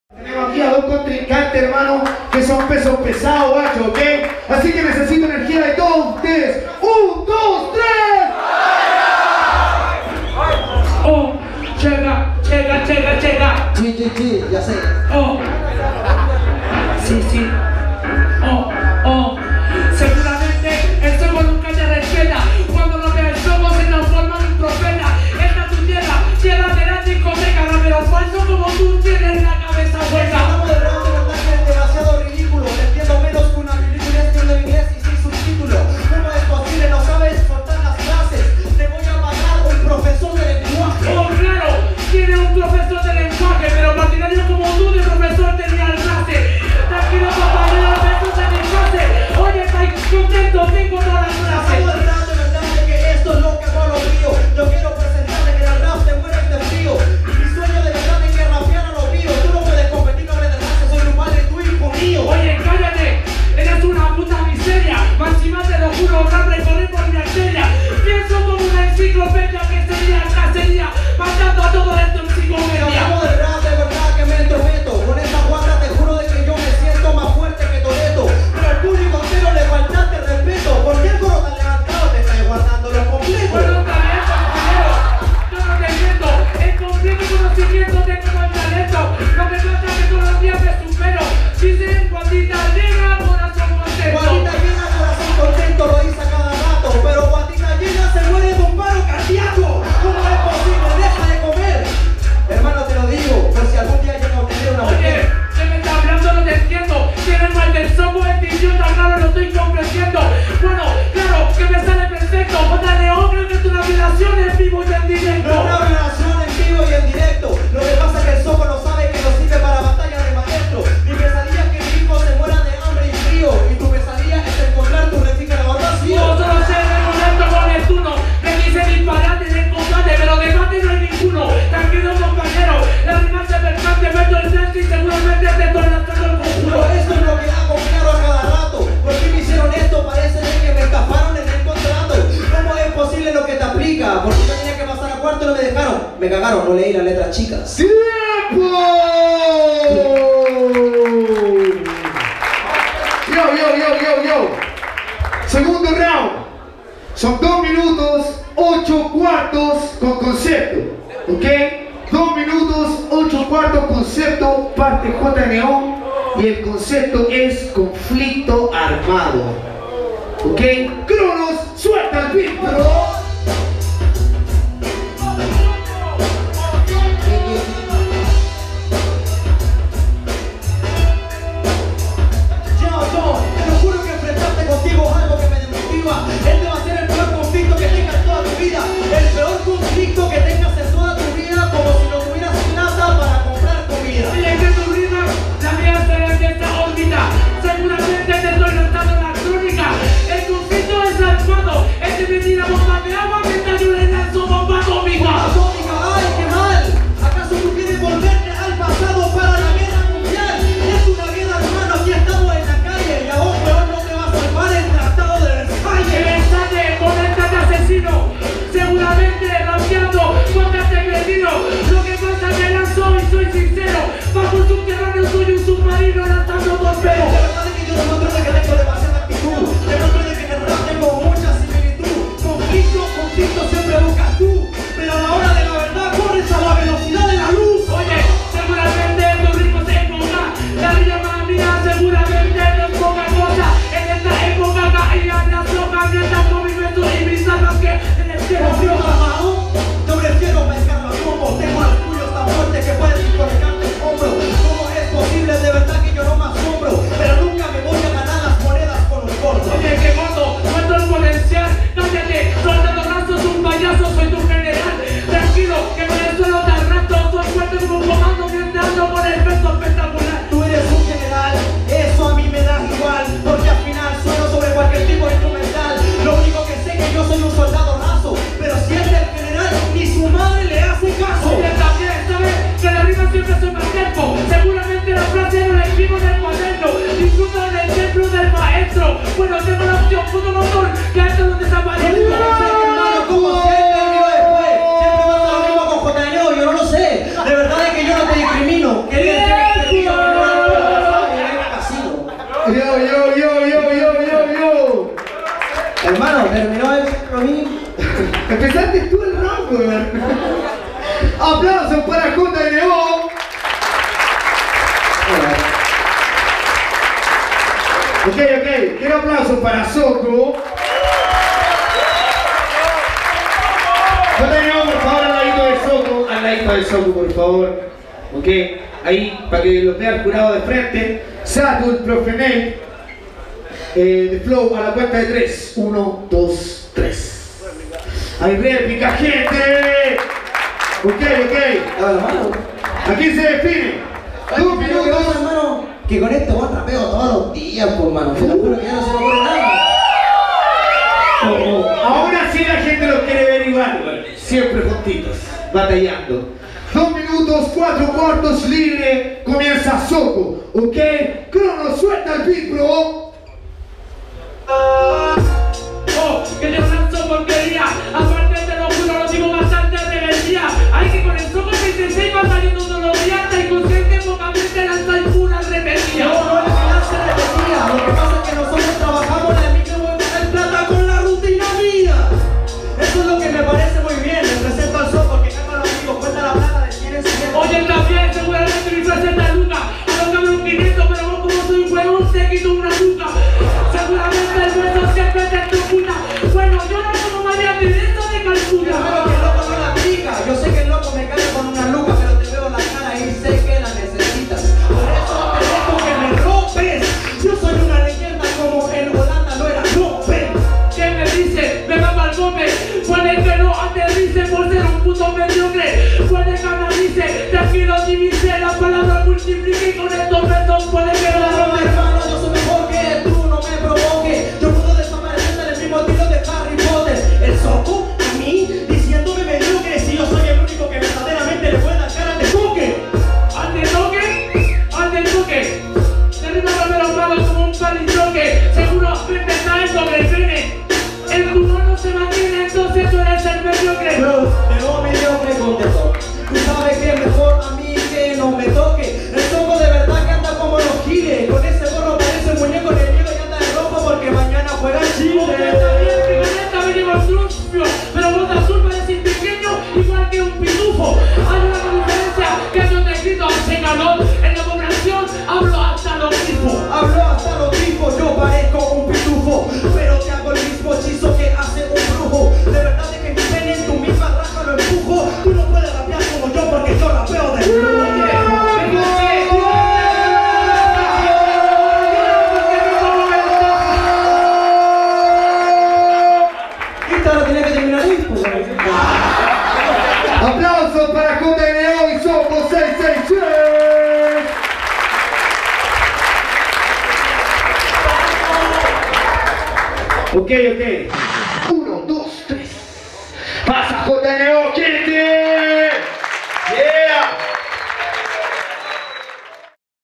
El Campeón del Templo 8º - JNO VS Soko666
El Campeón del Templo Torneo realizado por BATALLA DE MAESTROS, realizado en Espacio Dan Diego, Santiago el 25 de Junio del 2016.
Compitieron: Driser, Joqerr, Adesong, Deof, Eneese, Zpktro, Kylo, Anexo, Bicwan, pepegrillo, Efecto, Drefquila, Doble N, Yayo, JNO & Soko666.
De jurado estuvo: Satul, Prfe Neit & Deeflow.
Material grabado y editado por Catnegro Rec.

Material Exclusivo de Catnegro Rec. NO SE AUTORIZA A OTROS CANALES PARA EL USO DE ESTE MATERIAL.